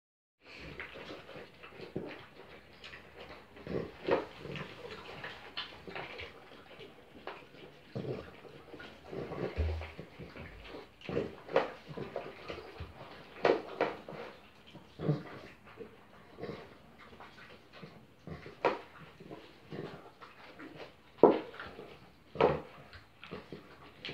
Bulldog francesi primo giorno di vita
I nostri piccoli Joker e Thor